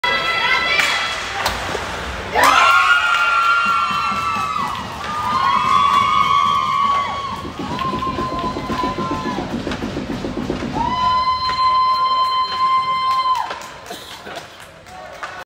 Owen’s goal
Score was incorrect, should be 1-0 Carshield at the time of this goal.